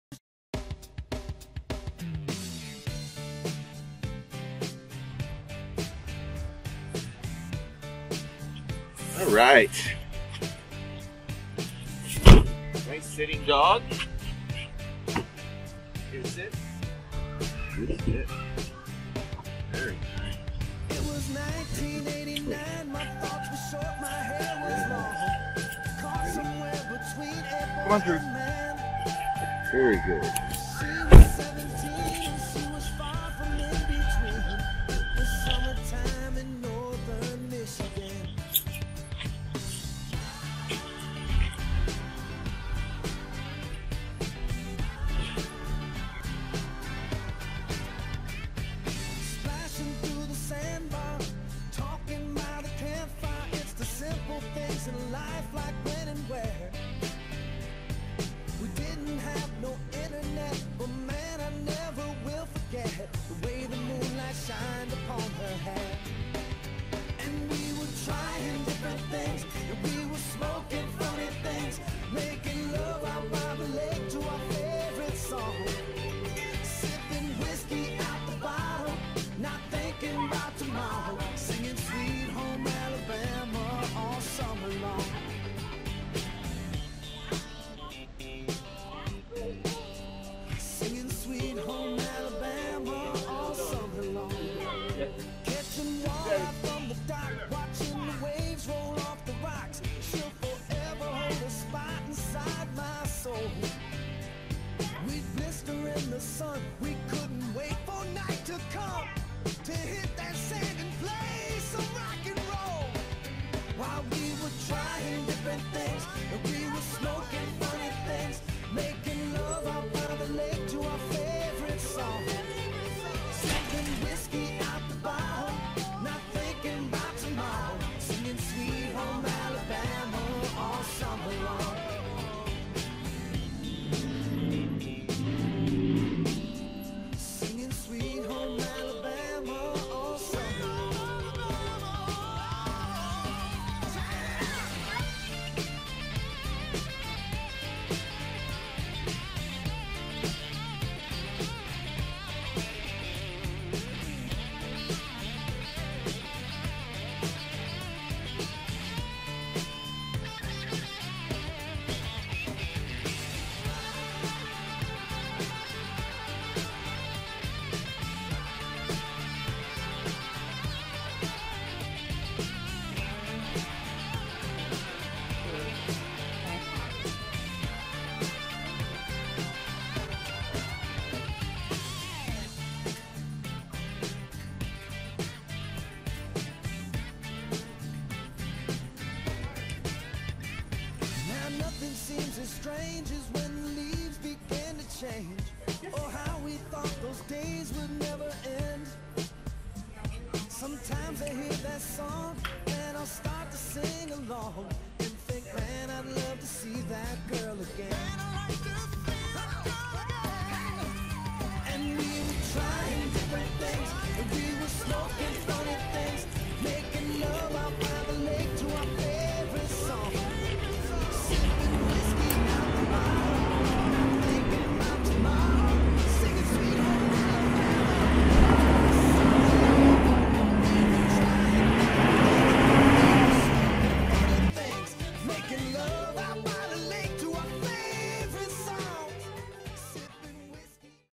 Tiana Earns an A Plus for 'No Kill'
Although still lacking focus, Tiana is turning out a really great result on outings these days...